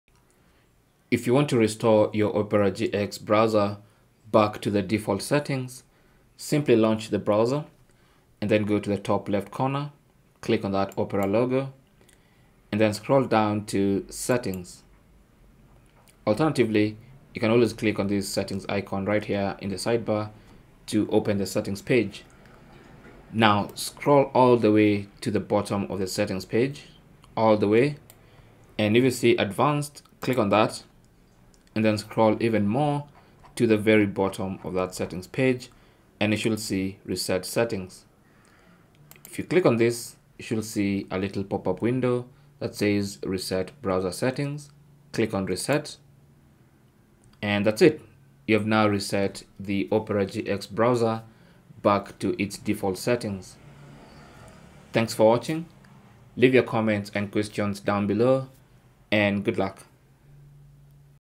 How To Reset Opera GX Browser Back To Default Settings | Restore Default Settings
Here's how you can reset Opera GX to default settings.
If you having issues with your browser, you can simply revert all the changes made back to the default. Maybe you might have changed something and it's interfering with your browsing experience and you can't remember how to change it back.

How To Reset Opera GX Browser To Default Settings
Launch Opera GX browser
Click on the 'Settings icon' on the left-side menu
Scroll to the bottom and click 'Advanced'
Scroll down to the 'Reset Settings' section
Click on 'Restore settings to their original defaults'
Click 'Reset' on the pop-up menu that appears

Cell Phone Tripod Adapter